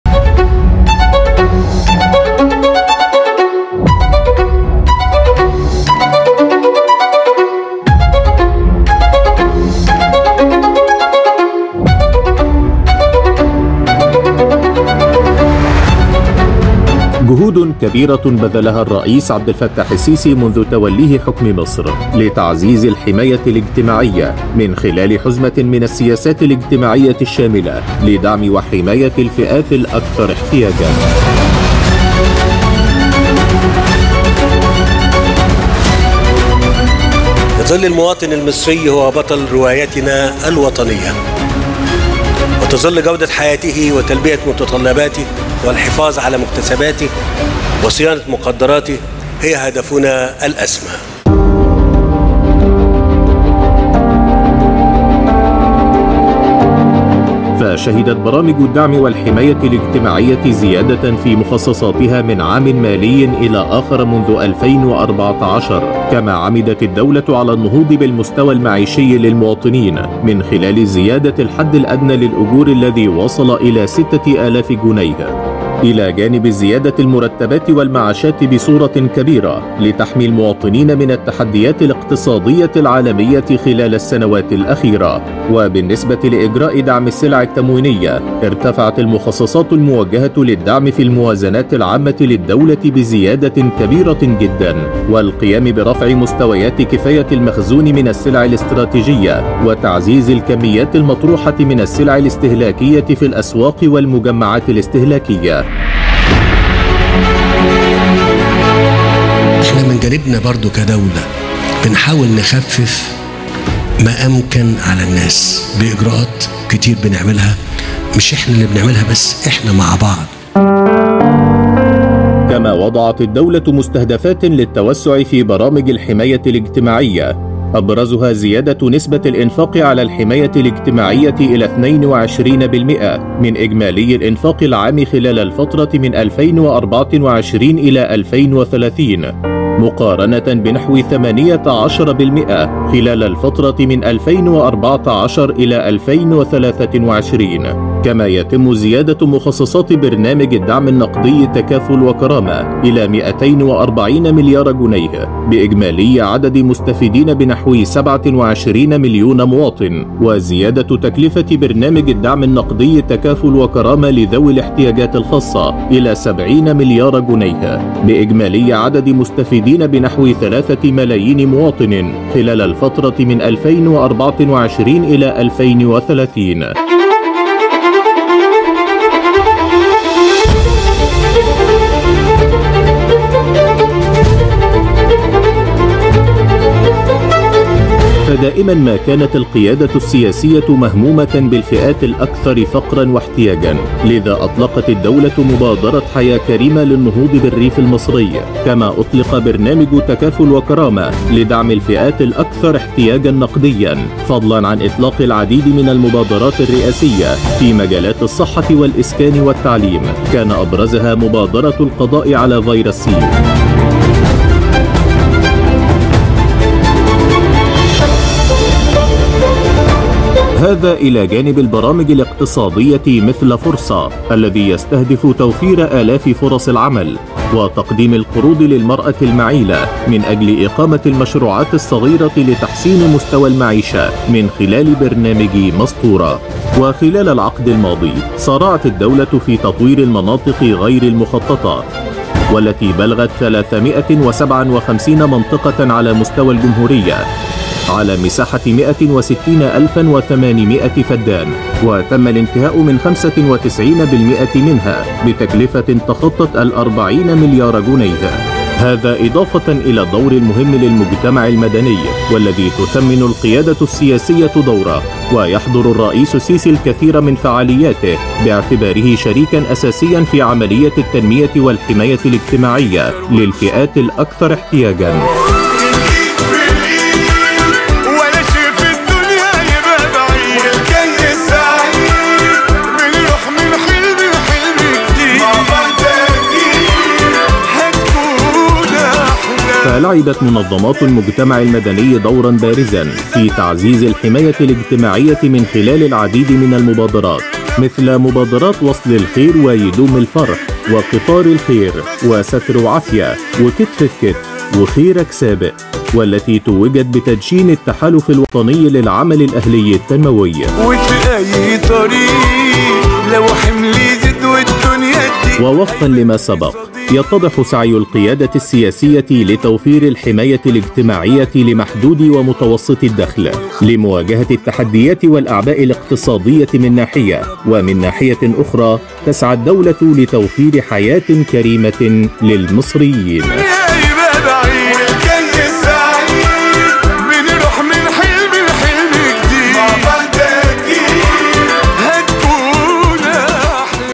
الدولة تعمل على تعزيز الحماية الاجتماعية من خلال حزمة من السياسات لدعم وحماية الفئات الأكثر احتياجا
الدولة المصرية تعمل على تعزيز الحماية الاجتماعية من خلال حزمة من السياسات الشاملة لدعم وحماية الفئات الأكثر احتياجا

29 مارس 2024